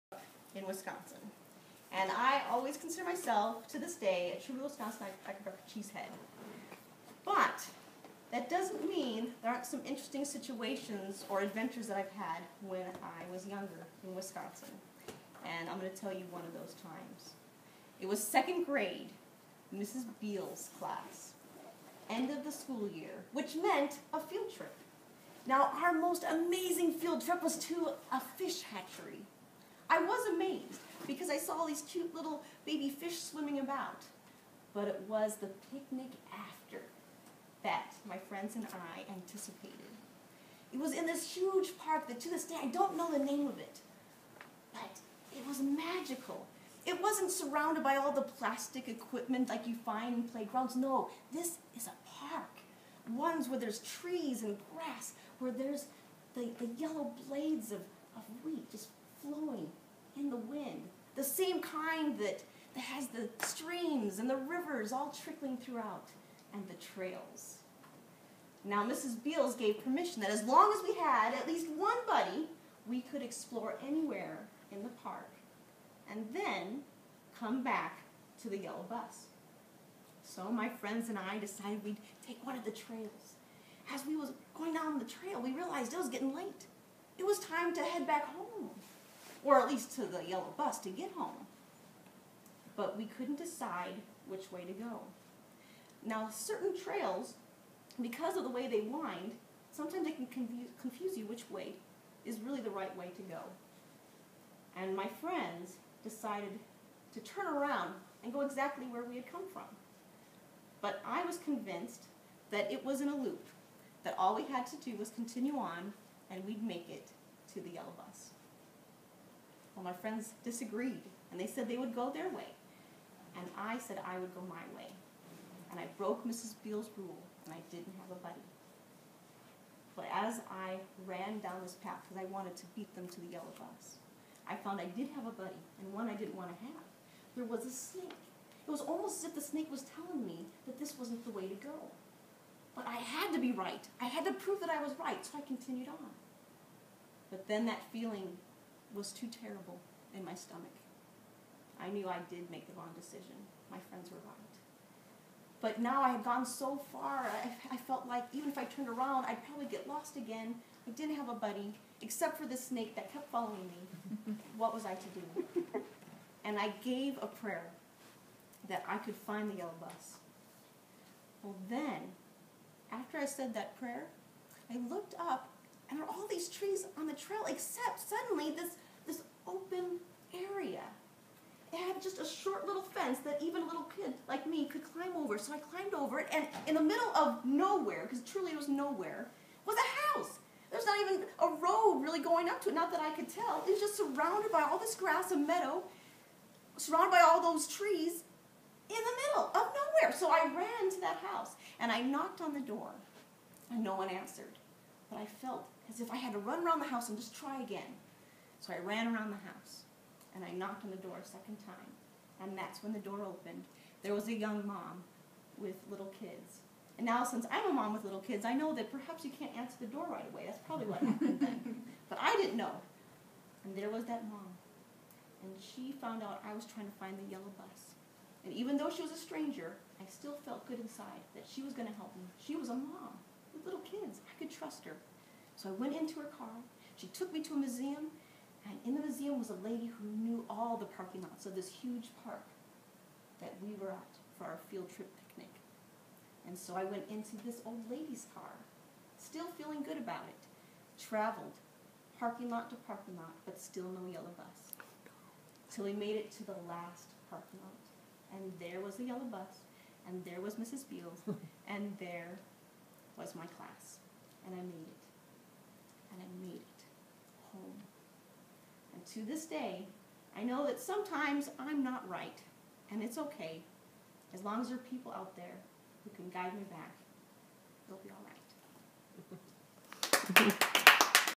Rachel's_Misadventures
Rachel Hedman--Personal story of childhood misadventures. 11.18.14